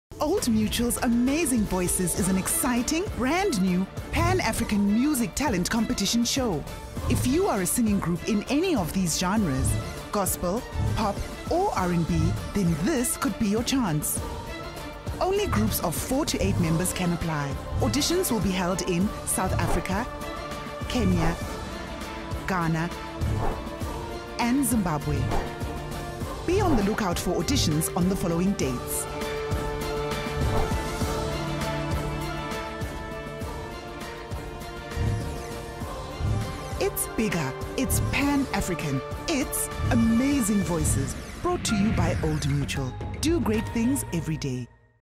The time is now to win $100,000 in the OldMutualAmazingVoices 🎤
Just form a musical group of 4-8 members and join the auditions on 1st October at the Kosados Arena Hotel, Kumasi and Accra, at the Christ the King Parish Hall on the 4th of October.